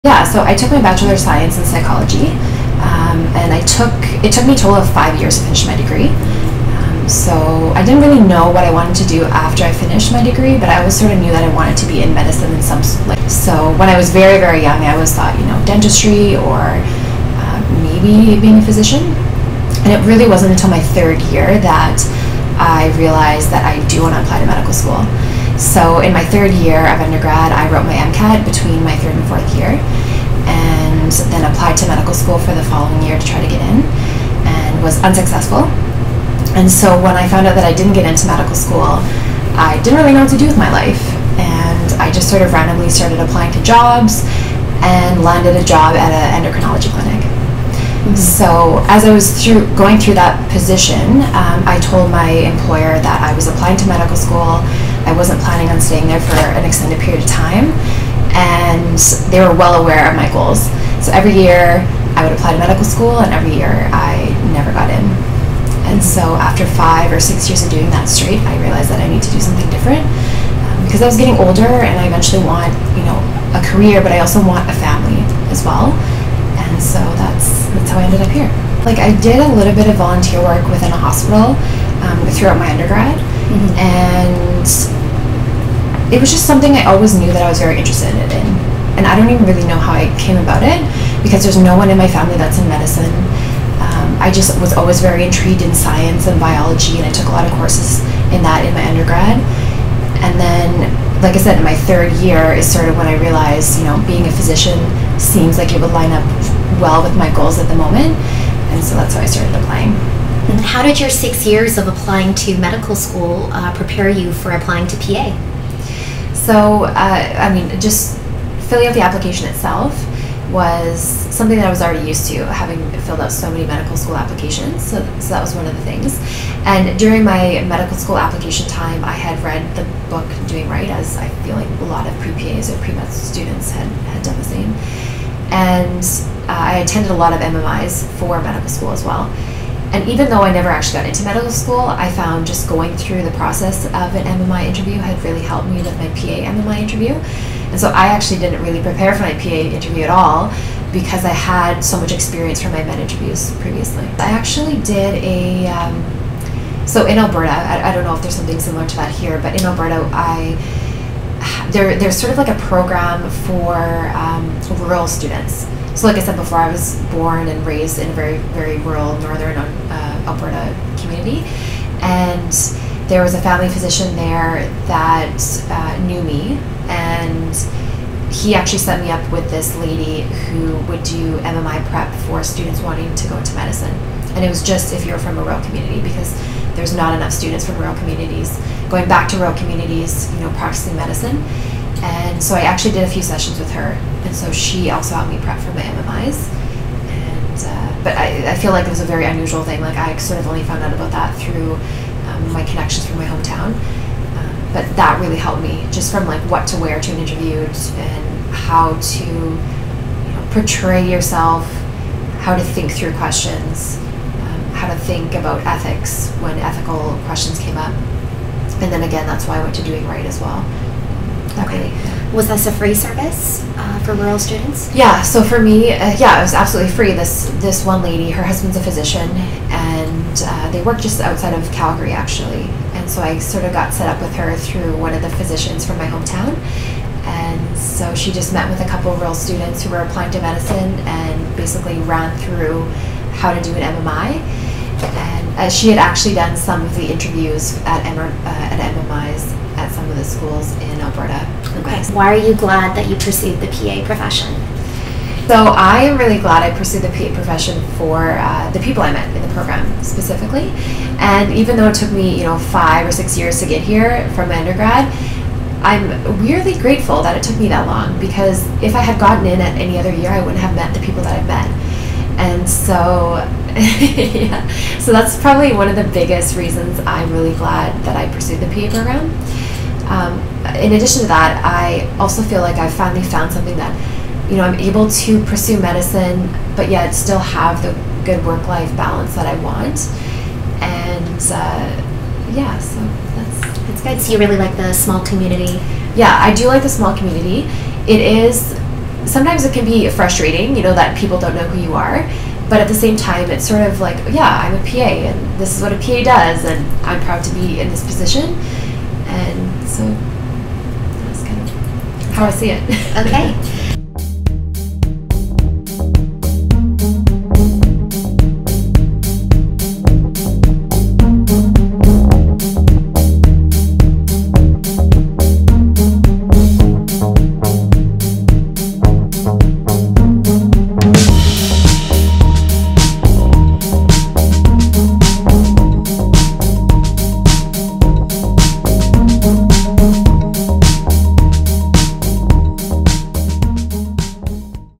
From Alberta to Ontario PA School - Kelsi's journey to PA School
Kelsi is originally from Alberta but successfully got into PA Consortium PA program in Toronto, Ontario. Here she explains her non-linear journey to PA School.

❐ JOIN US ON OUR ON OUR FREE FB GROUP ❏